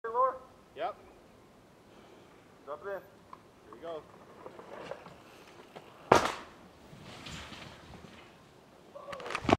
huge whistler gap
gaping over the trail at whistler, this drop was in new world disorder 6